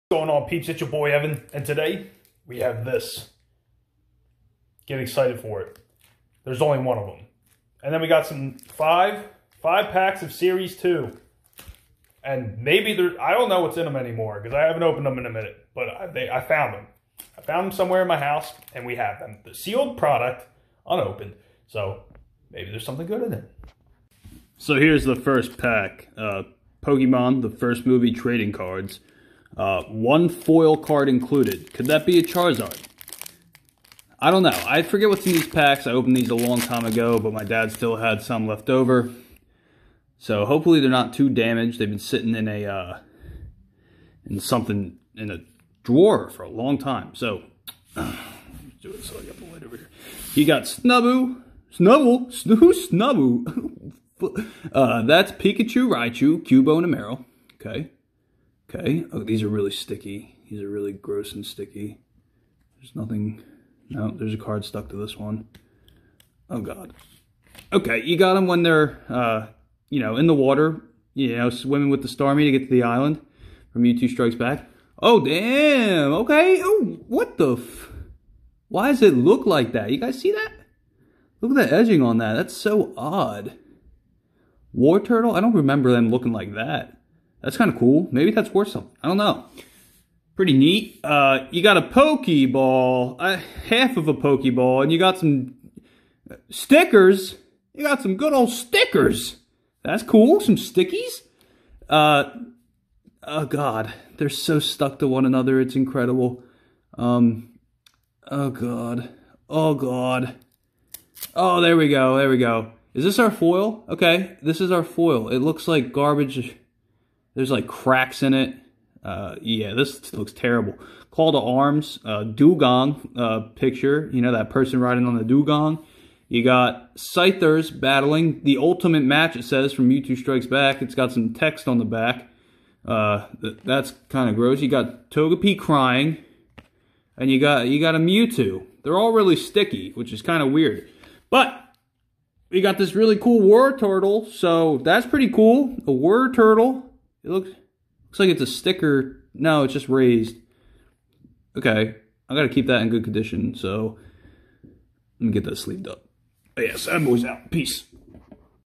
Opening a Pokemon TOPPS The First Movie Pack!
Opening some vintage TOPPs pokemon cards!